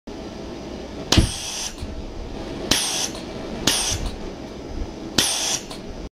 Pneumatic fireball - special effect
Creating a fireball using a 3% oil and gasoline mixture (oil is for the tiny fire balls and extra smoke). The fluid was turned into fog usign a 230VAC pneumatic valve and a 6.3 BAR compressor. Everithing was ignited with a propane torch.